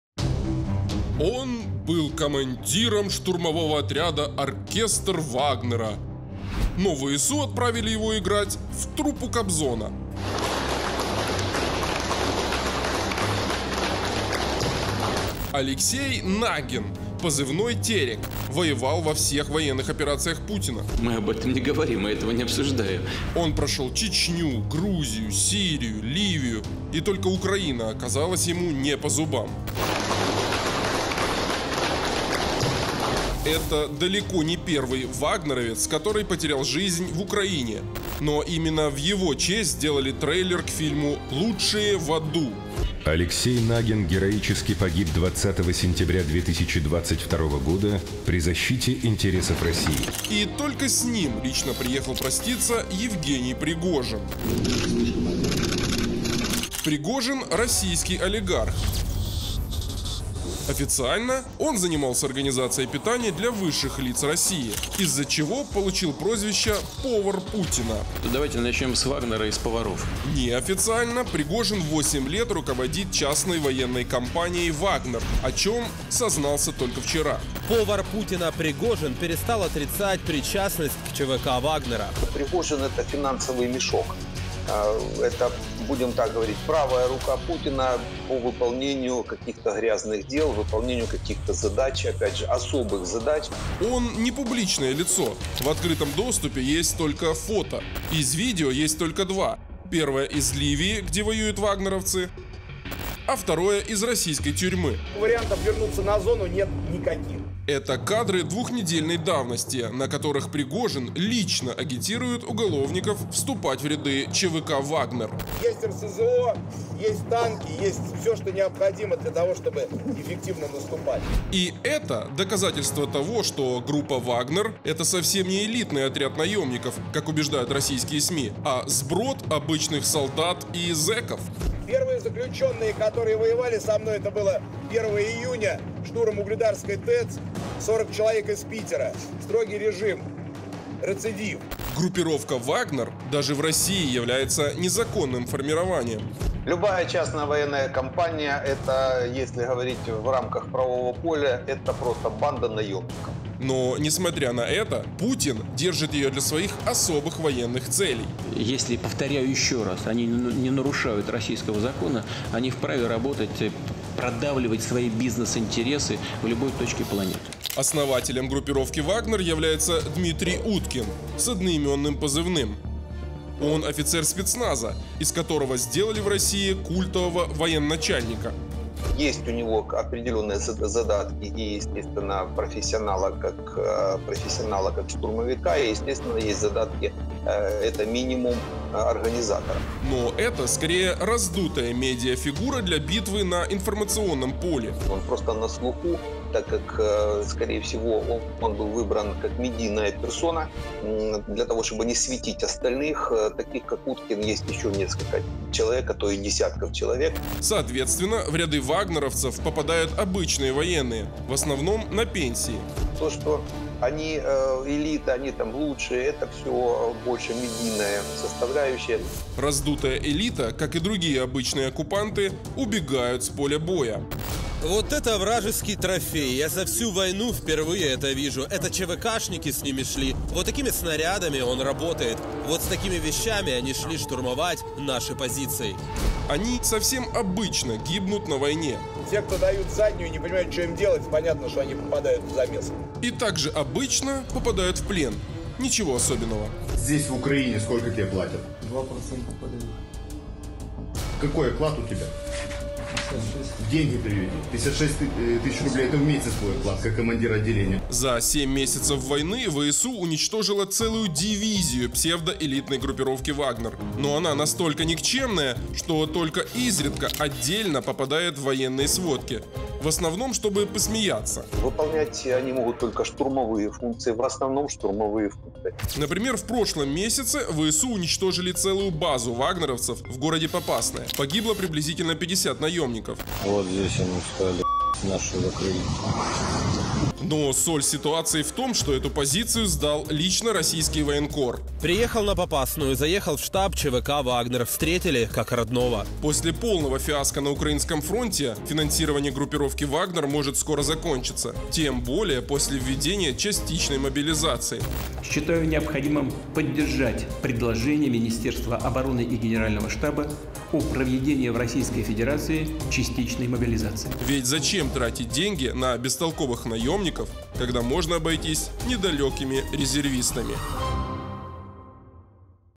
☠️Командир отряда ЧВК "Вагнер" уехал домов в пакете
24 сентября Анатолий Штефан, полковник ВСУ, сообщил об очередном демилитаризованном "вагнеровце". На войне в Украине погиб Алексей Нагин с позывным "Черкес"/"Терек". Нагин – командир штурмового отряда "Терек, подразделения ЧВК "Вагнер". Чем еще знаменит жтот утилизированный? Детали в сюжете!